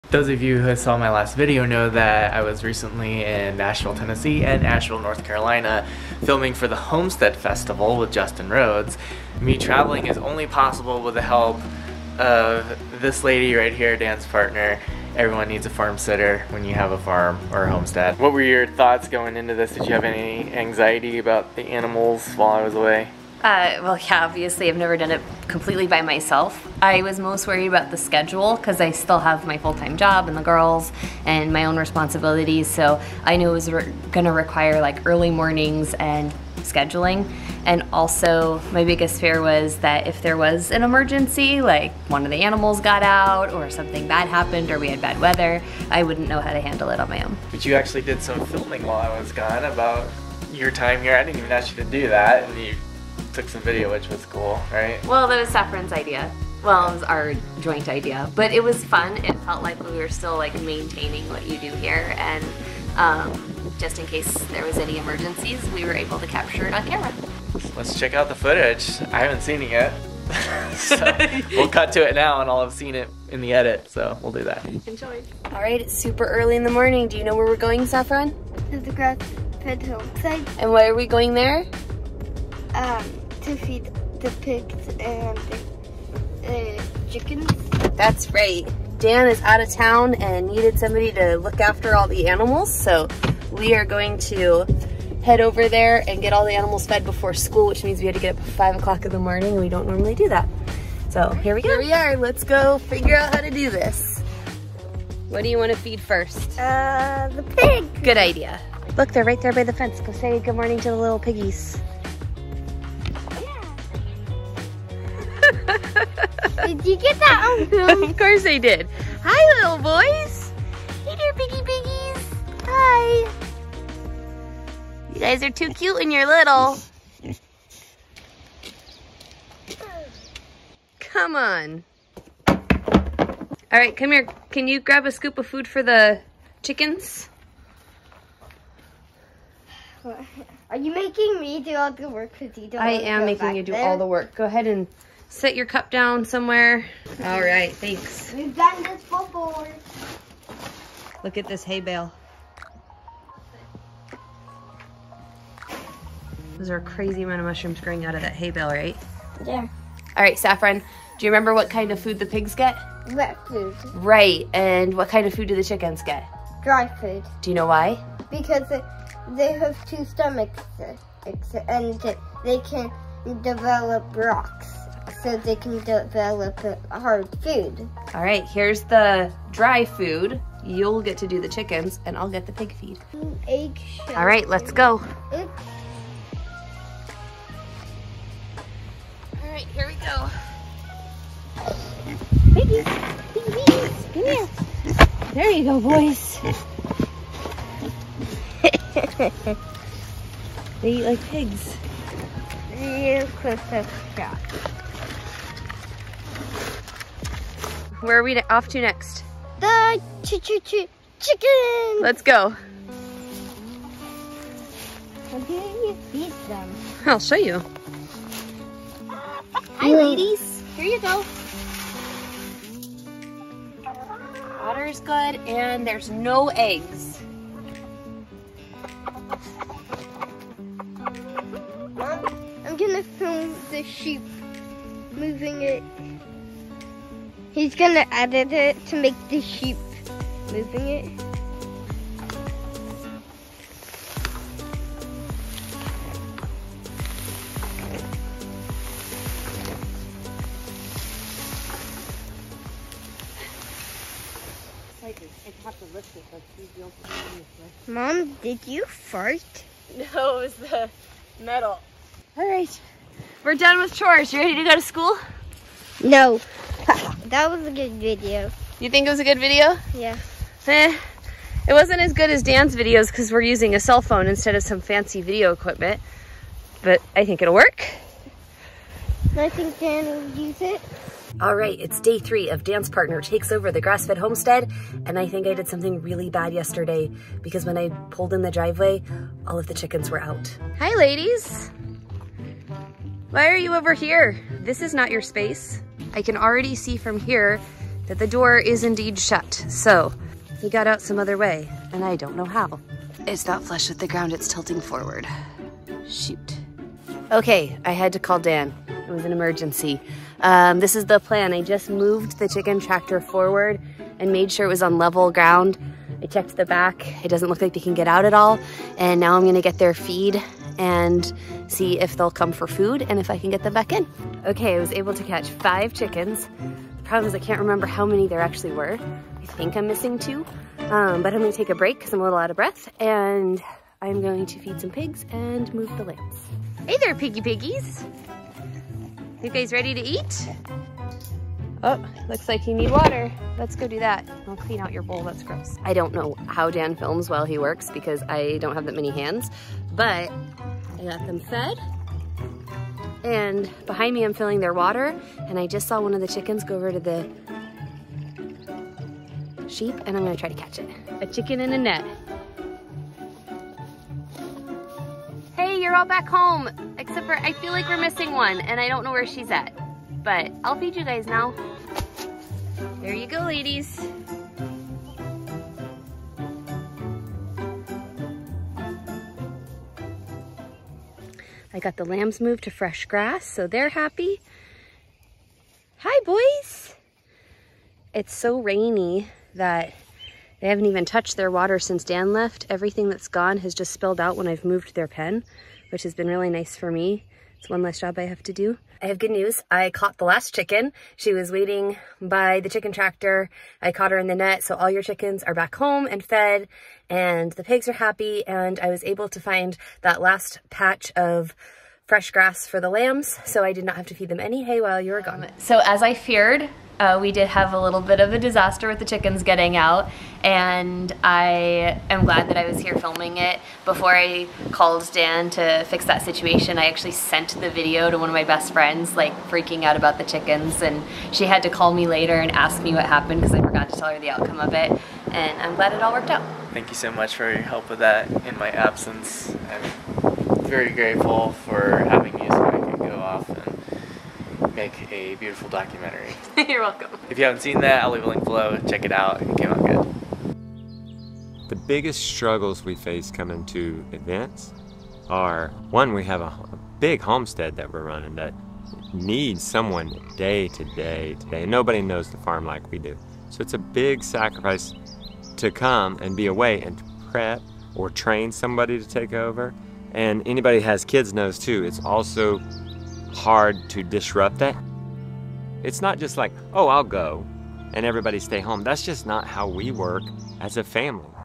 She Ran My Homestead for 5 Days
Dance Partner took over the homestead while I was away in Asheville and Nashville.